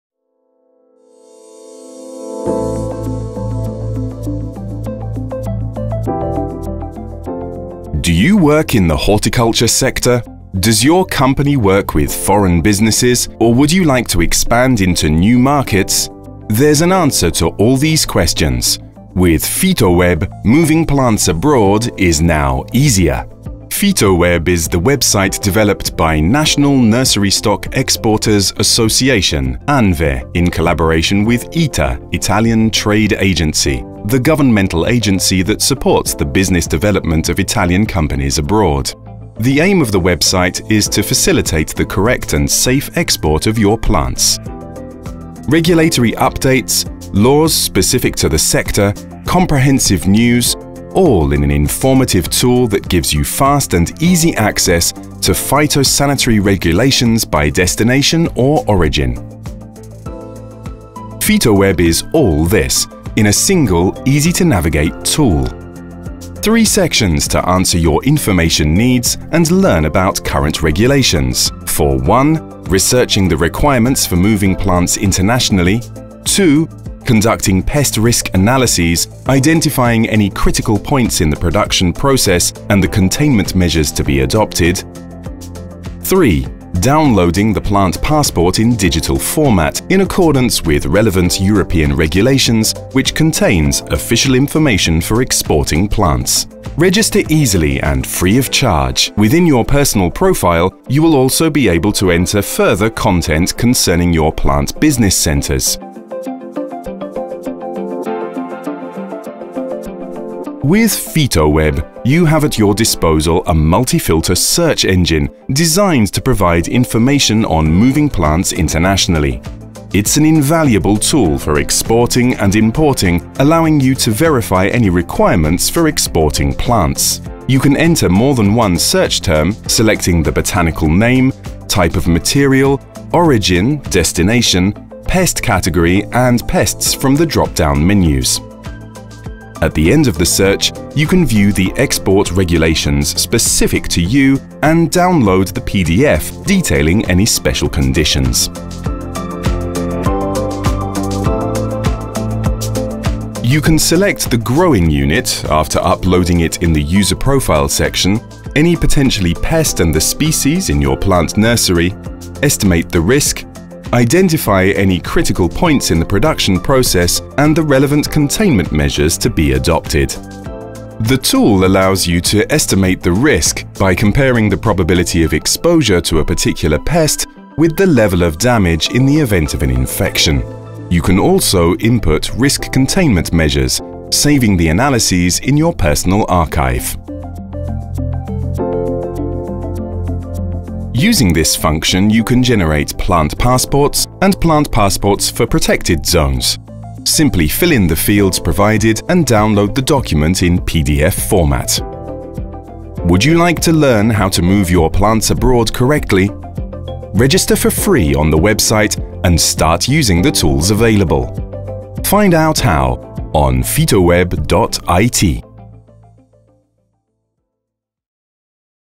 PHYTOWEB.IT
Phytoweb is the project, developed by Italian Trade Agency (ITA) and ANVE, representing a groundbreaking innovation in the horticultural sector. Phytoweb is a dedicated portal for plant management, providing users with easy and immediate access to phytosanitary requirements. This portal is completely free and only requires the creation of a user profile.